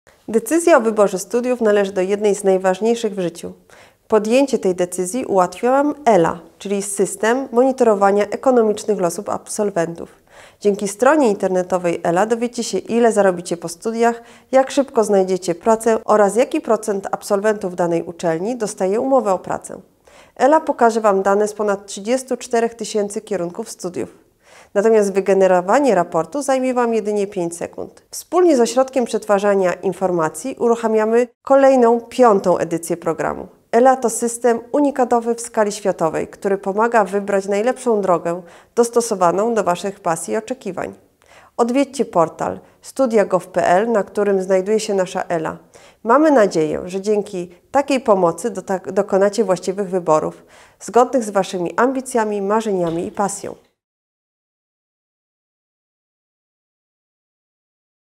Startujemy z 5 edycją Ekonomicznych Losów Absolwentów!
ELA to system dostarczający sprawdzonych informacji na temat sytuacji absolwentów uczelni na rynku pracy w Polsce. Teraz możemy zapoznać się z najnowszą wersją systemu ELA.